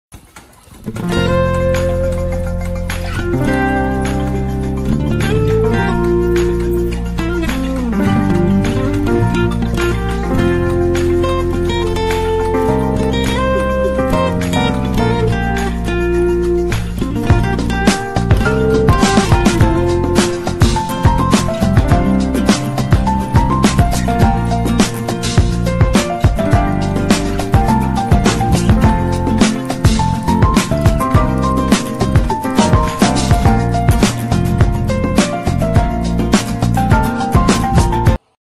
automatic mask earloop welding machine with auto feeder
Name Automatic mask ear loop welding machine
Function blank mask to ear loop mask
Speed 100-110pcs/min
Lifting ear rope diameter 2.5-3.5
Use materials Ear rope+ mask sheet
Way of working Pneumatic + electric
Air compressor power 0.5-0.8mpa
Total power 8.6kw
Power supply 220v
Center distance 75
Mask sheet size 175*95mm
Welding method 20K 2000W Ultrasonic welding
Packing Size 208*125*155cm